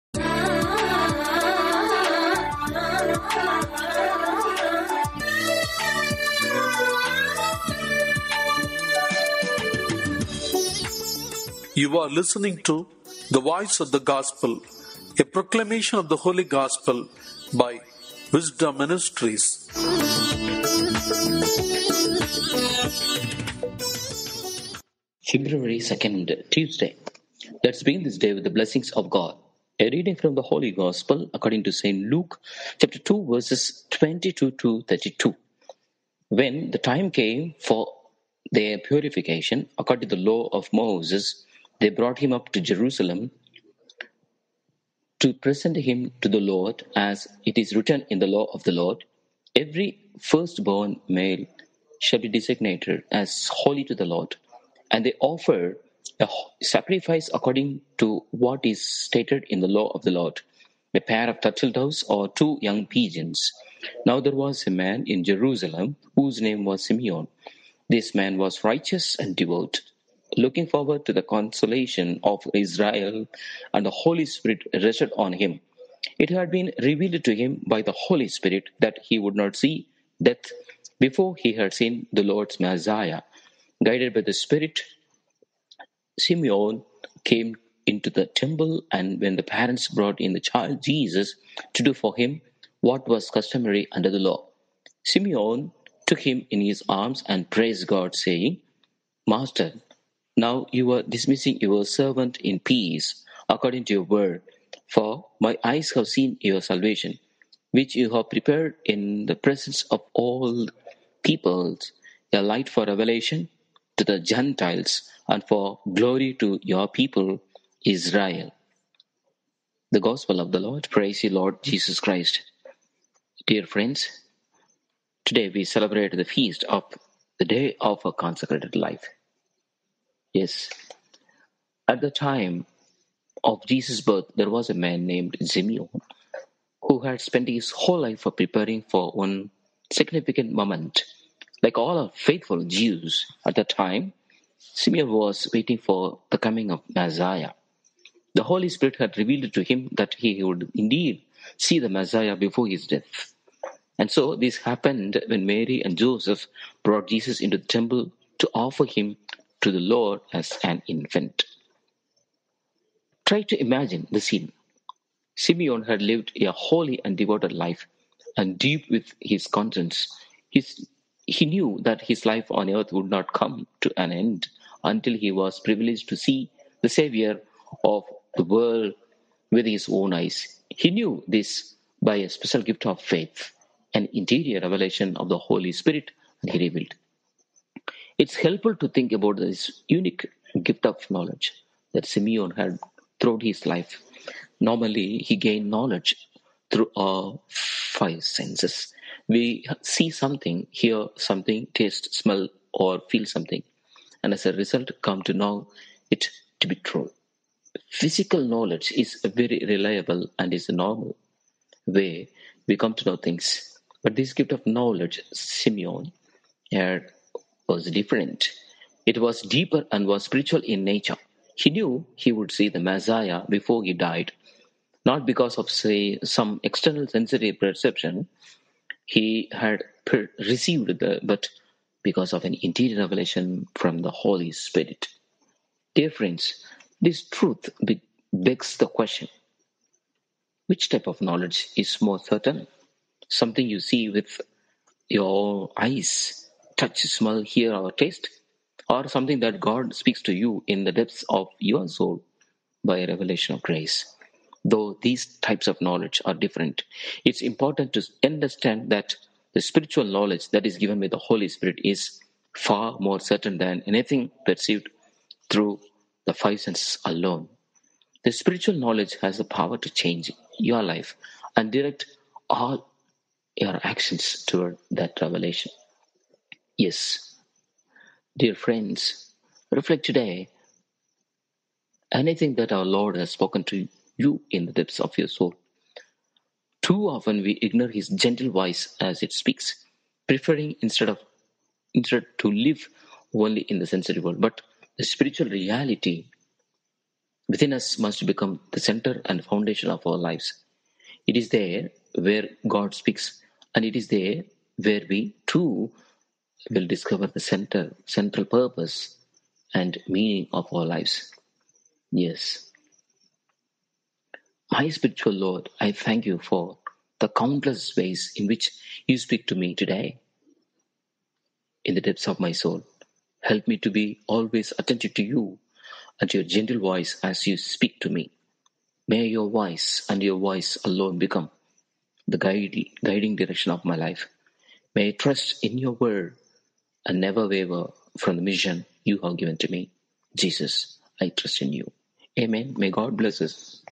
TheVoiceoftheGospel -2.2.2021-Tuesday PresentationoftheLord Ordinary_Time_YearB_Week4 Lk_2:22_40
Jesus offered himself to us expecting nothing.
That is the divine love.